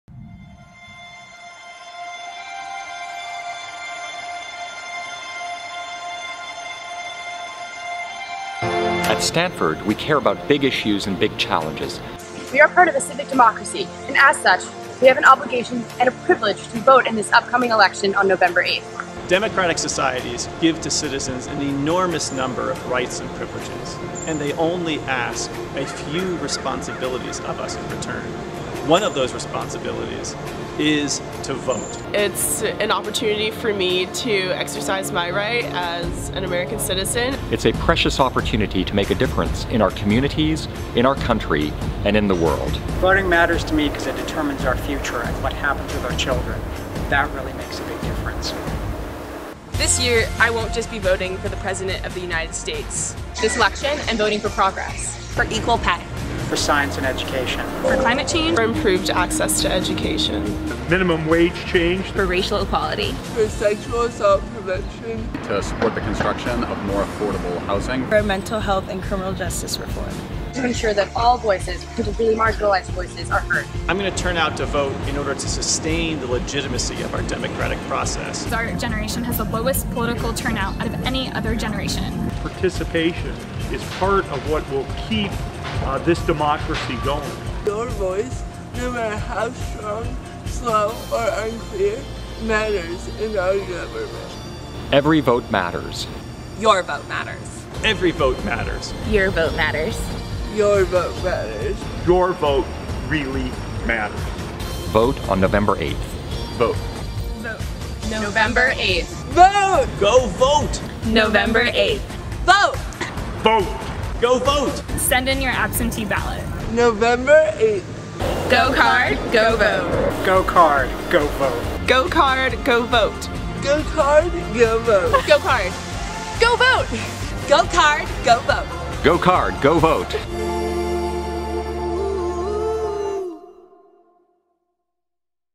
SIG: Get Out the Vote!
Requests for absentee ballots must be received by Nov. 1 in California!